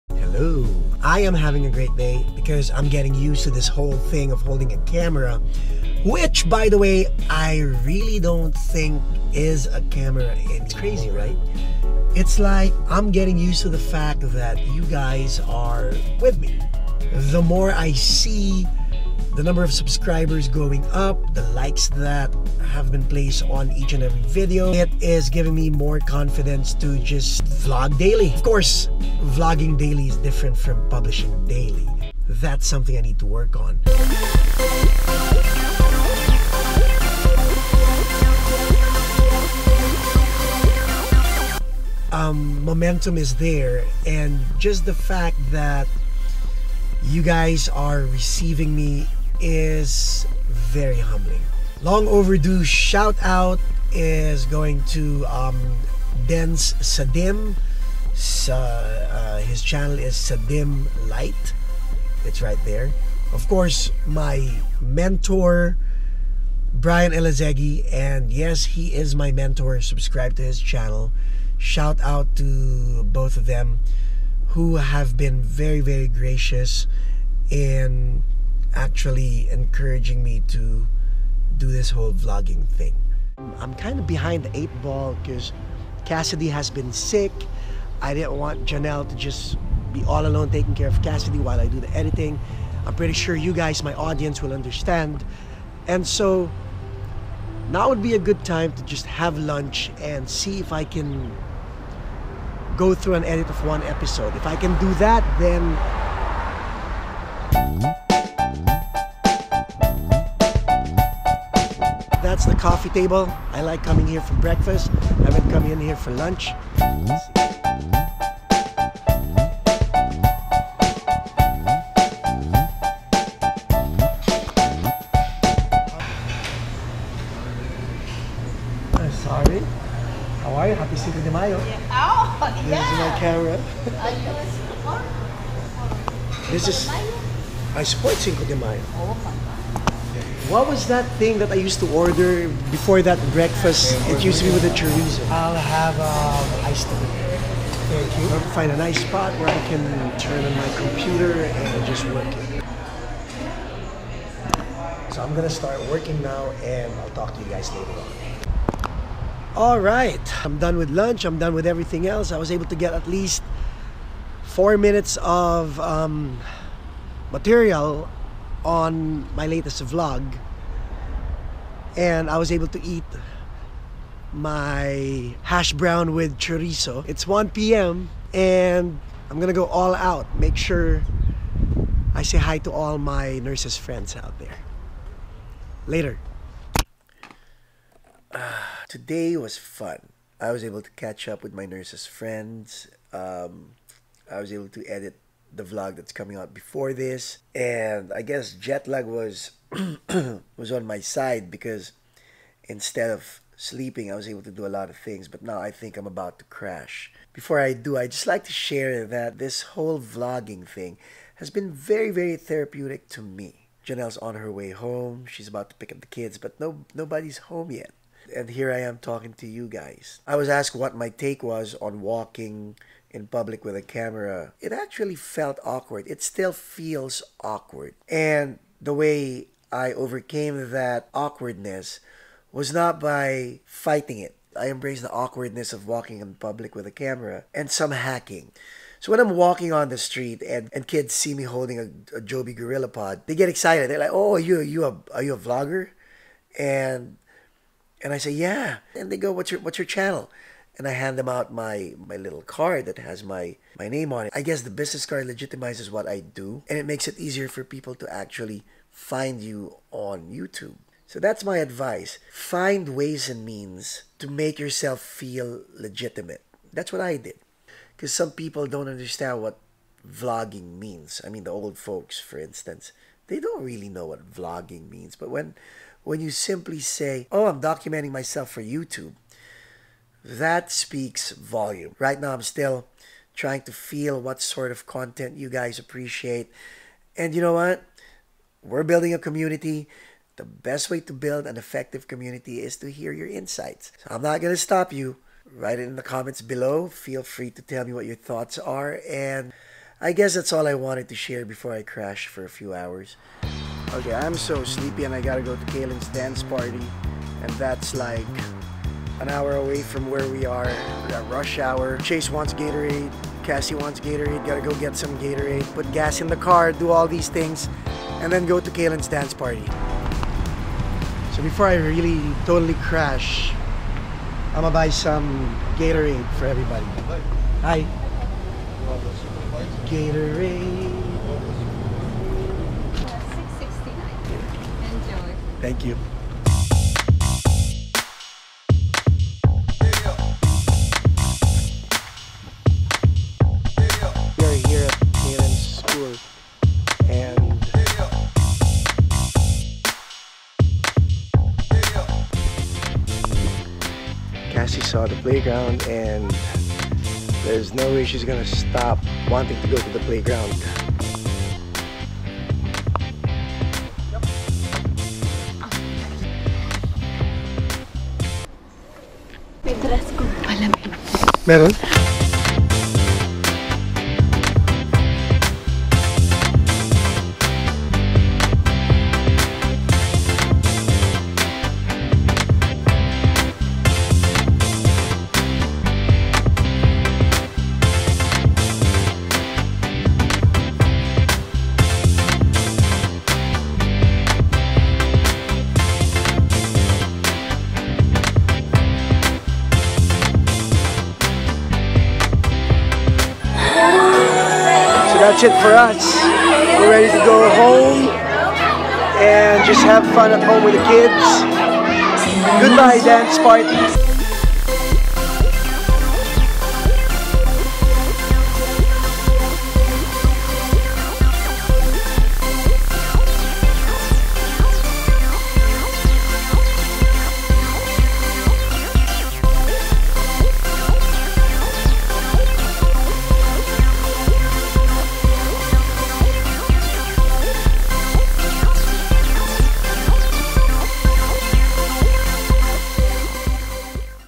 LIFE IN THE PUBLIC EYE - Vlog#29
Vlogging in public is NOT easy, specially if you are in a dense area. People are curious about you and what you're doing and they tend to stare... most of the time

In this vlog, I share with all of you how to vlog in public without feeling awkward about it. I also let you in on a little hack I do to create a more relaxed environment for me and for the people who are in the area I am vlogging at.